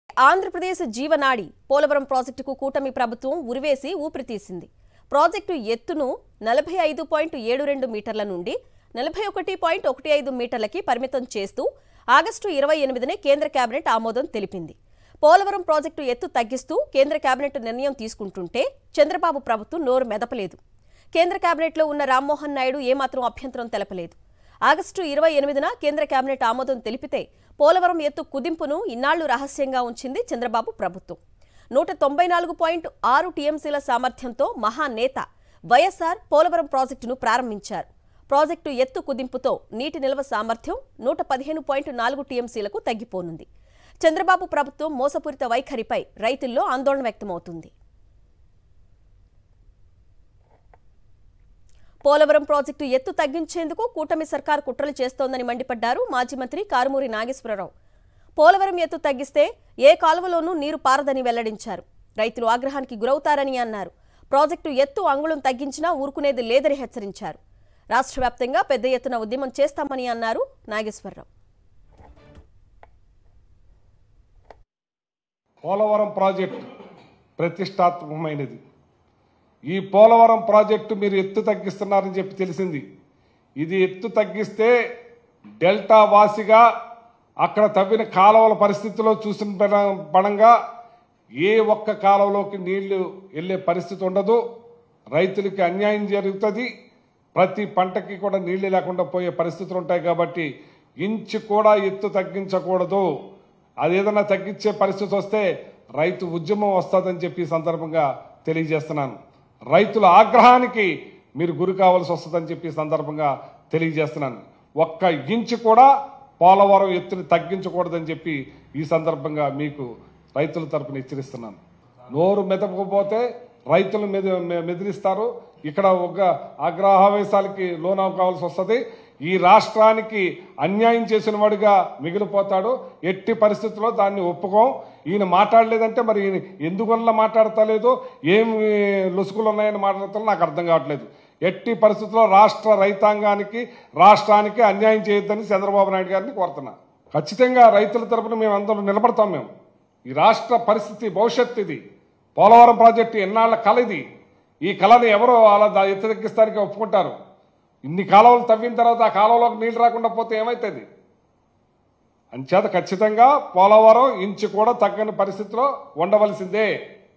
Karumuri Venkata Nageswara Rao Fires on AP Government Over Polavaram Project
పోలవరం ఎత్తు తగ్గిస్తే ఊరుకునేది లేదు
కూటమిపై కారుమూరి ఫైర్